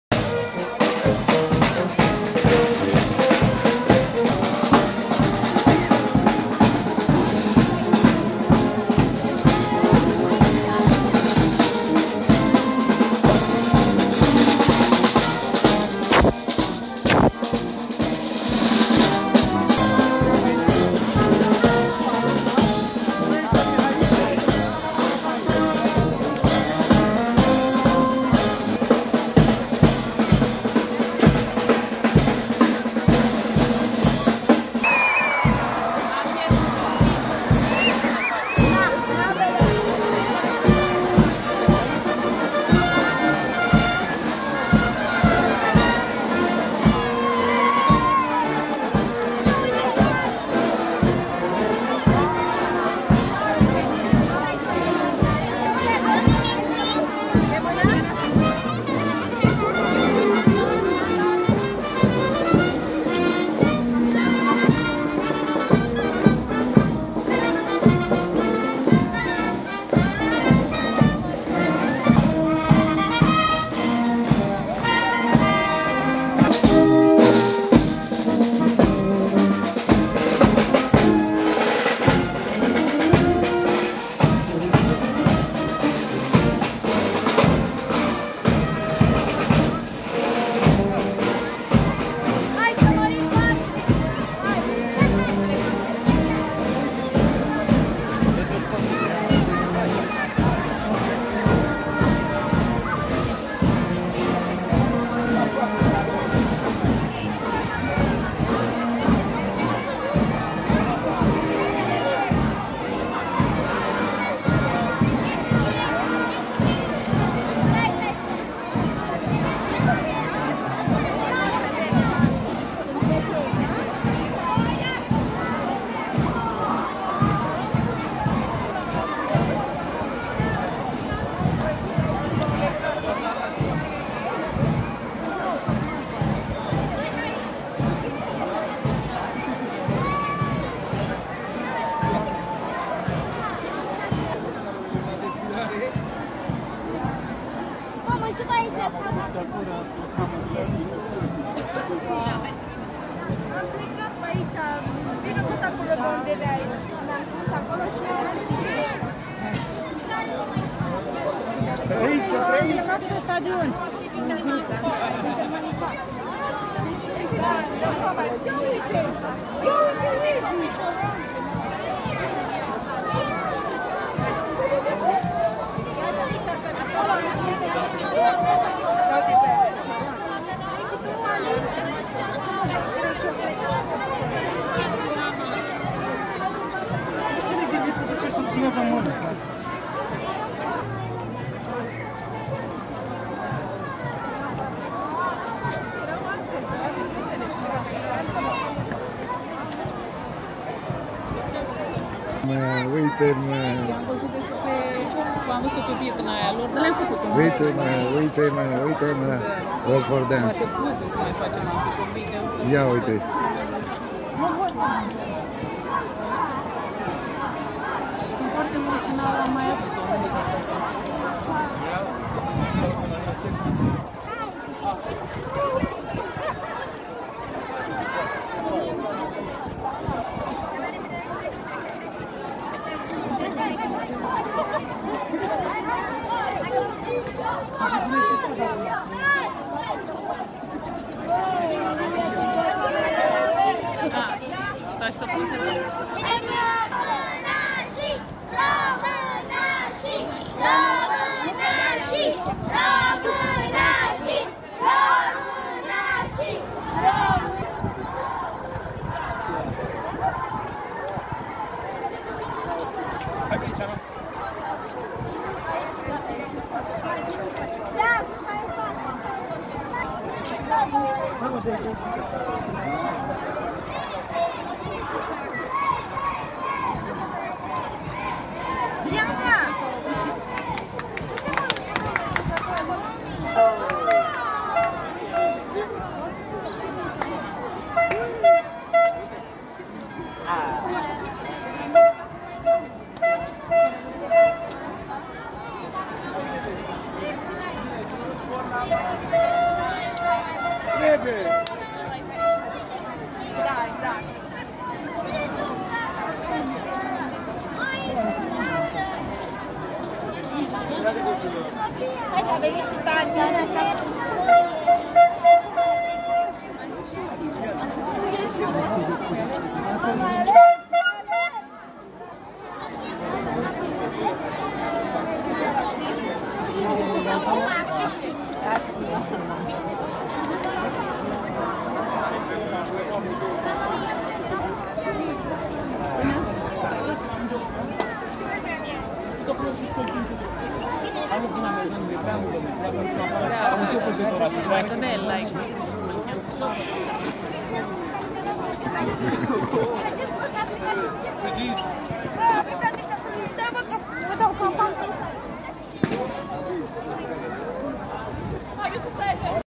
Targoviste- Parada Copiilor 1 iunie 2024
Coordonator Mălina Andrei, organizator Teatrul Tony Bulandra si Primaria Targoviste